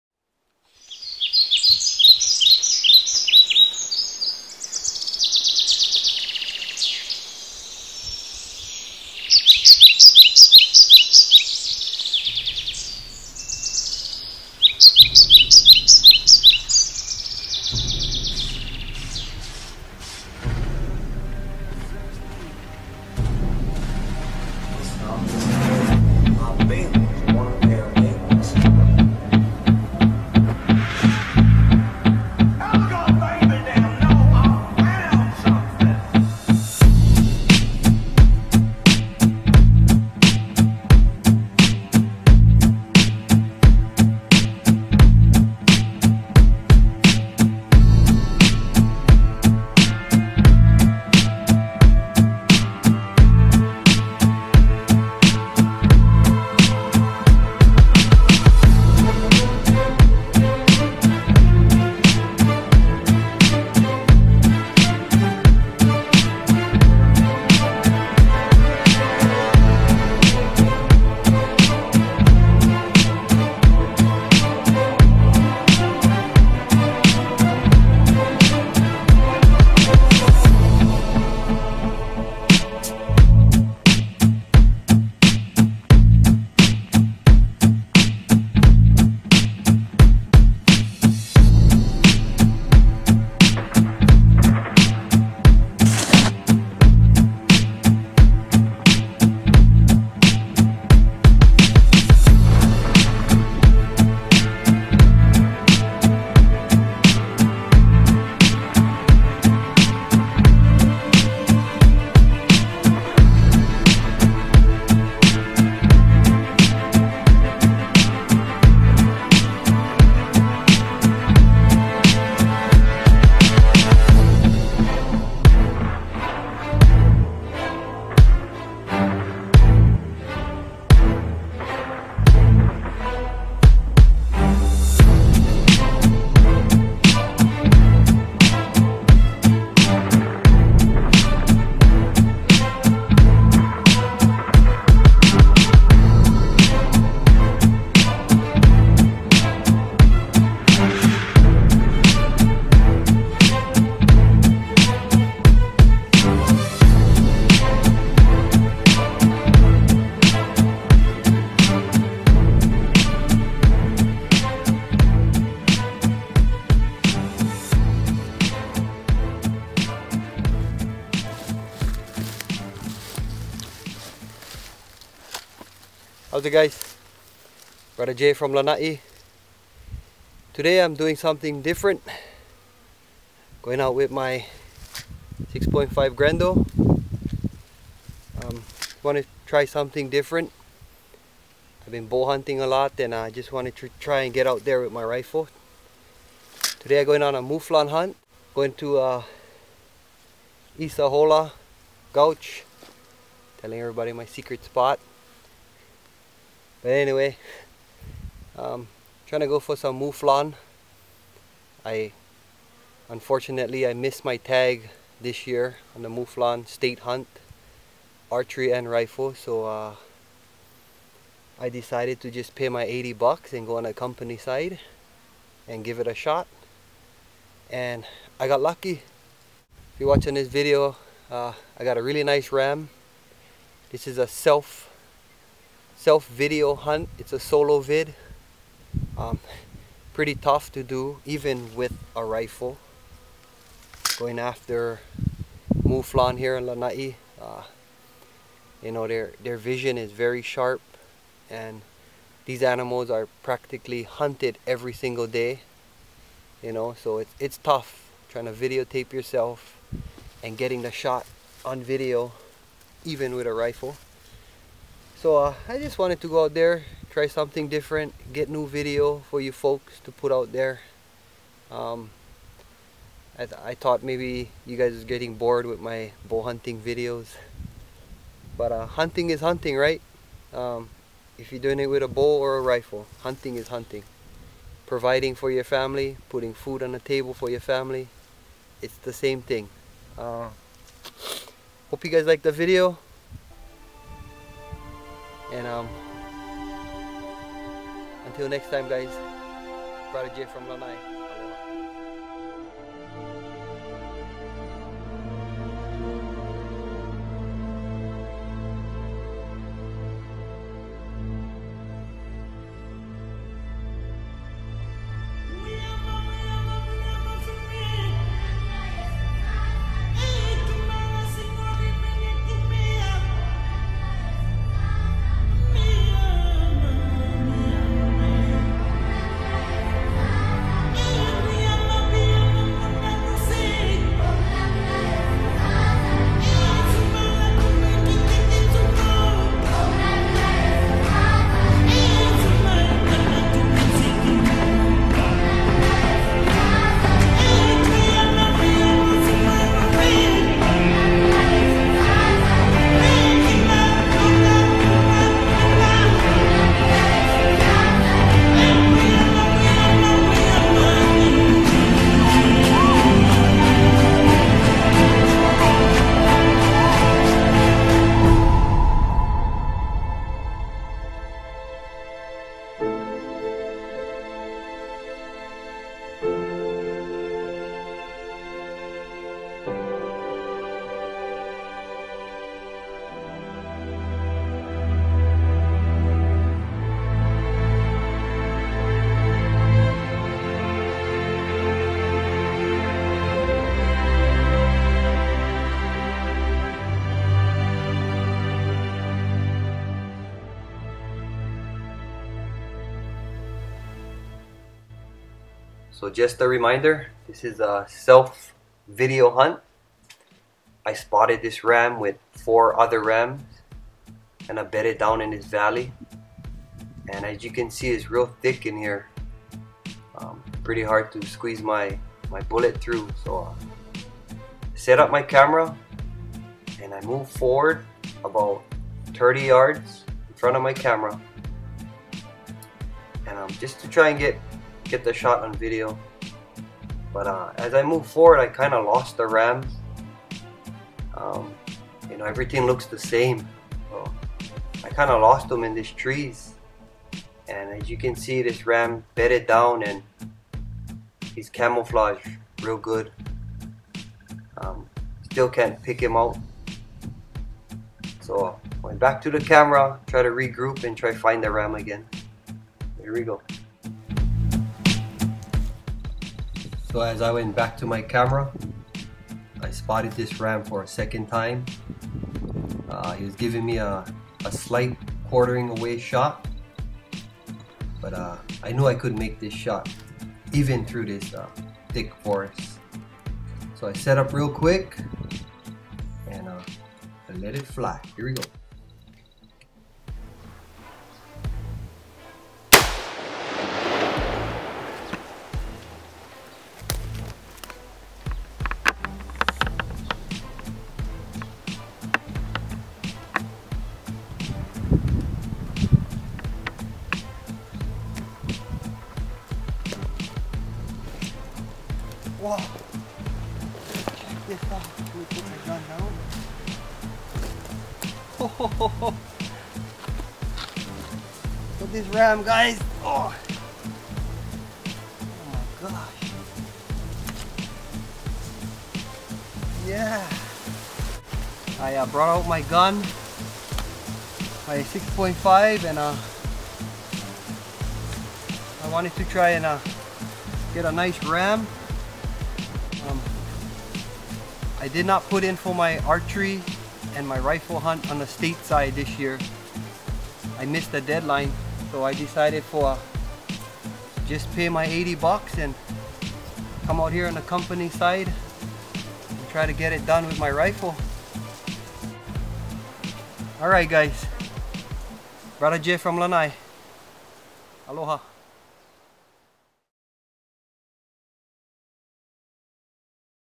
2014 Mouflon Hunt Lana'i Hawaii
Warning this video has graphic scene...this is my 2014 Mouflon hunt. I  did not put in my tags for 2014 state hunt. so I planed a hunt on the company side.